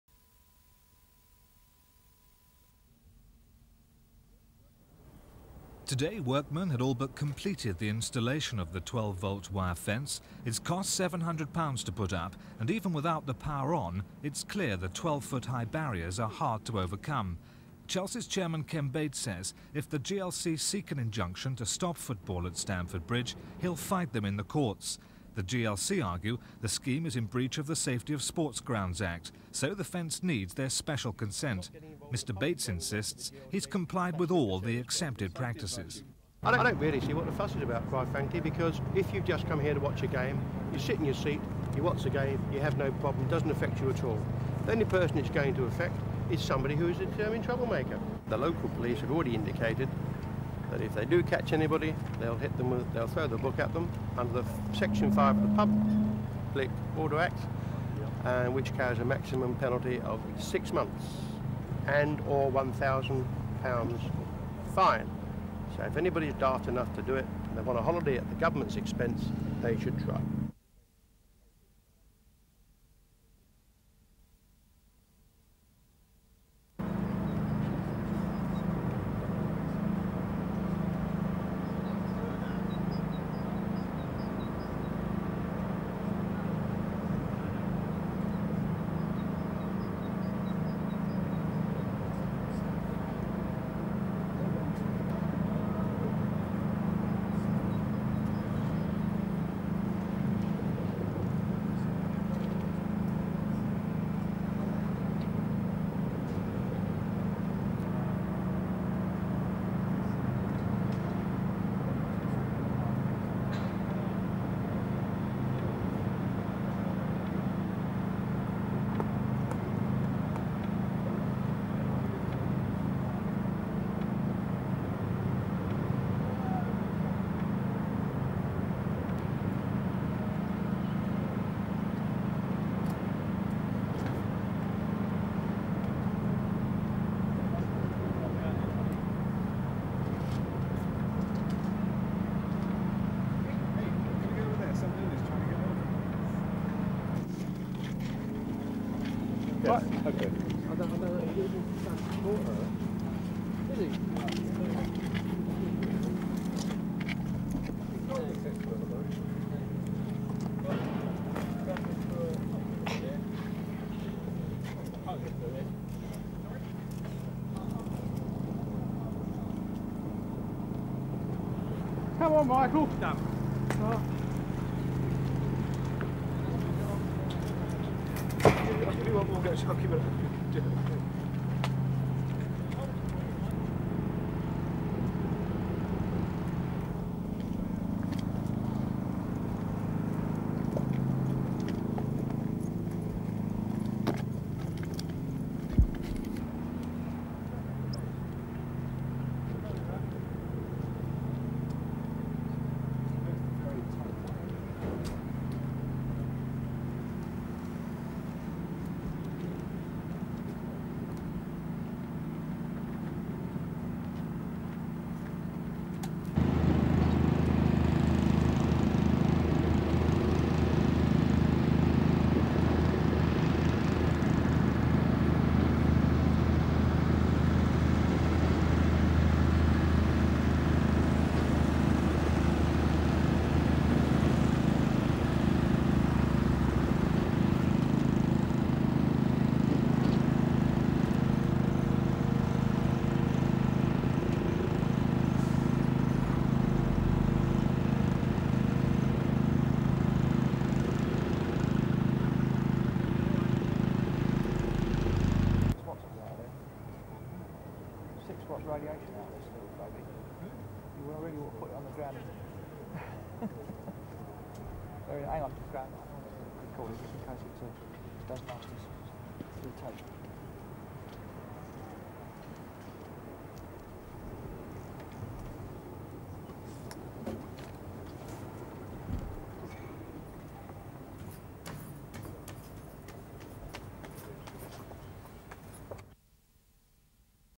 Chelsea Football club | Install Electric Fence | Shocking Fans! |Ken Bates| TN-85-062-007
Ken Bates say it like it is when talking about why he installed electric fencing.
over zealous fans may find it shocking!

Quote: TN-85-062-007